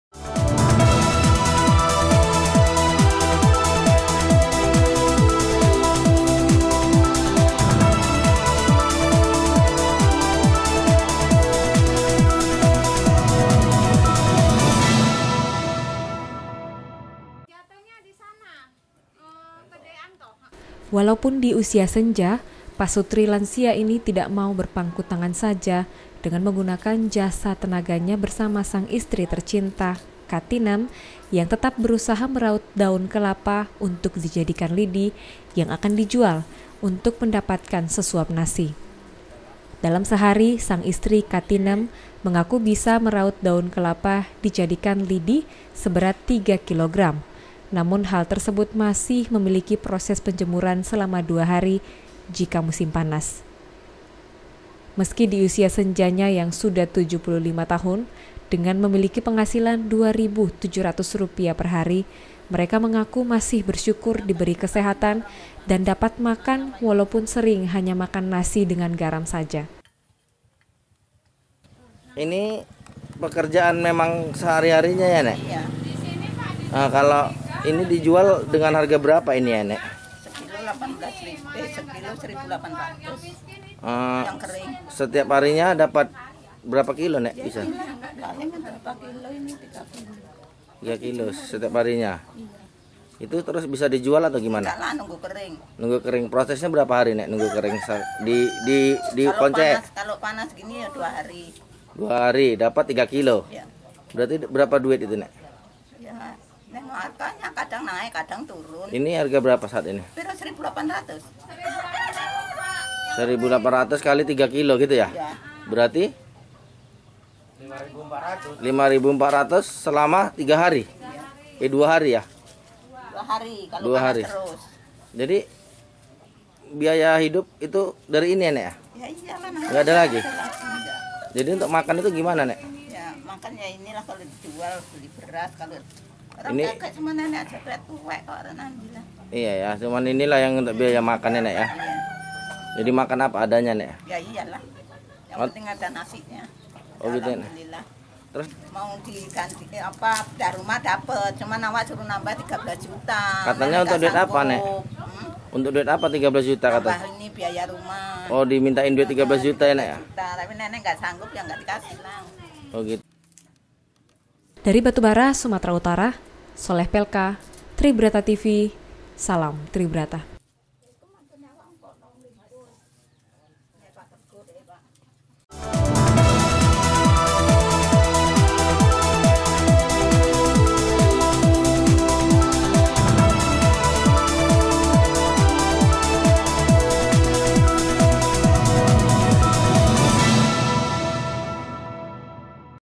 POTRET BURAM WARGA MISKIN DI BATUBARA PASUTRI PERAUT DAUN KELAPA
Walaupun di usia senja pasutri lansia ini tidak mau berpangku tangan saja dengan mengguna sisa tenaganya  bersama sang istri tercinta katinem yang tetap berusaha meraut daun kelapa untuk dijadikan lidi, yang akan dijual untuk mendapatkan sesuap nasi.

Dalam sehari sang istri  katinem mengaku bisa meraut daun kelapa dijadikan lidi seberat tiga kilo gram, namun hal tersebut masih memiliki proses penjemuran selama dua hari jika musim panas.

Meski diusia senja nya yang sudah 75 tahun dengan memiliki penghasilan 2700 perhari, mereka mengaku masih bersyukur diberi kesehatan dan dapat makan walau sering hanya makan nasi dengan garam saja.

Dari Batubara Sumatera Utara Sholeh Pelka Tribrata Tv salam Tribrata.